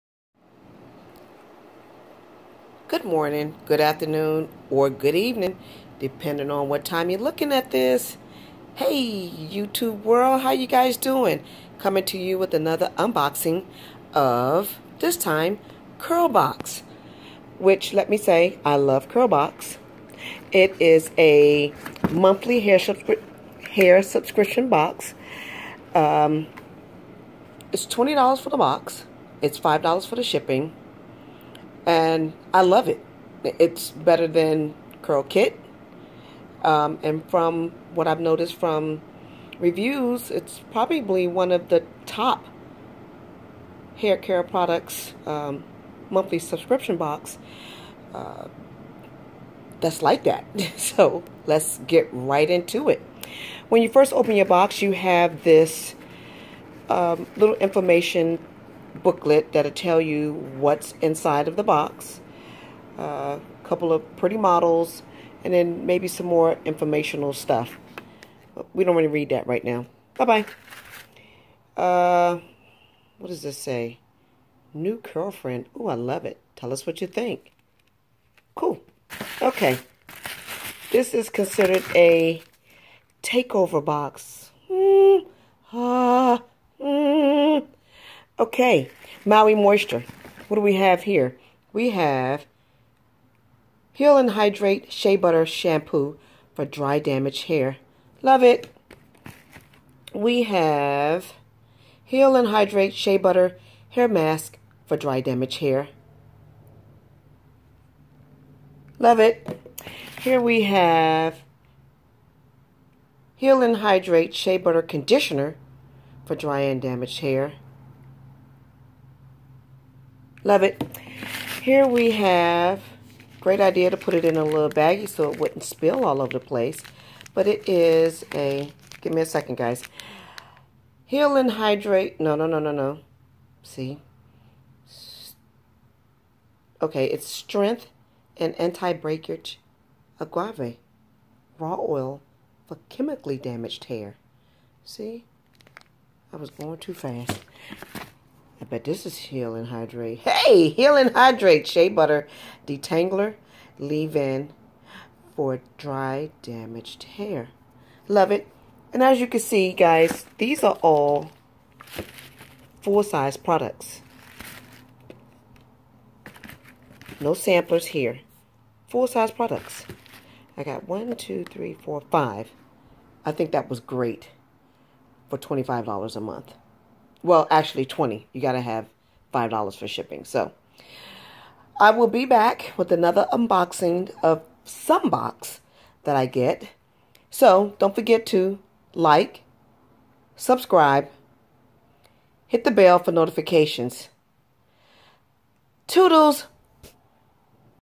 "Unboxing" June Curlbox 2018
This is a monthly subscription box, the cost is 20.00 plus 5.00 for shipping.